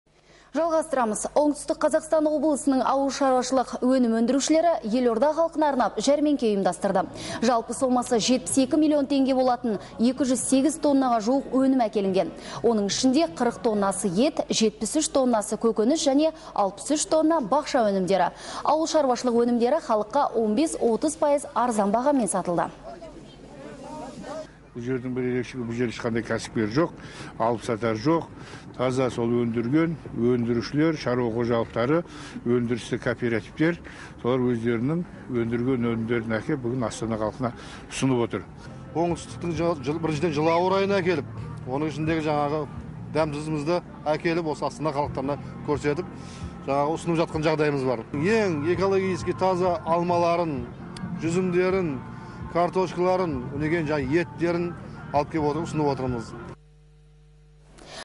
Елорда халқына арналған жәрмеңке ұйымдастырылды
Жәрмеңкеге жалпы сомасы 72 миллион теңге болатын 208 тоннаға жуық өнім әкелінген.